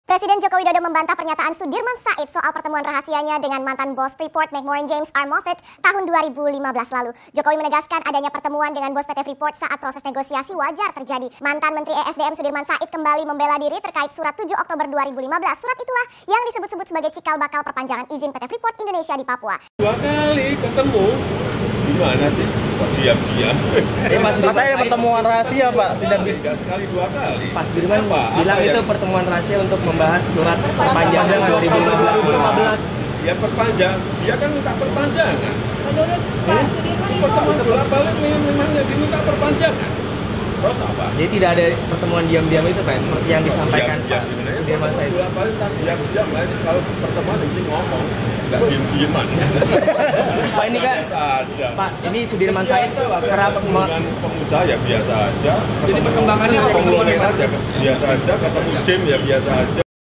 Sudirman Said Bohong Soal Pertemuan Jokowi dengan Bos Freeport
Sudirman Said menganggap adanya pertemuan rahasia antara Jokowi dengan bos Freeport. Padahal, yang dilakukan Jokowi tidak rahasia. Bahkan pertemuan tersebut dilakukan secara berkali-kali. Hal yang aneh jika dikatakan rahasia. Sejak mendukung Prabowo, Sudirman suka berkata bohong. Mungkin itu tujuan dari kubu Prabowo. Menebarkan hoaks.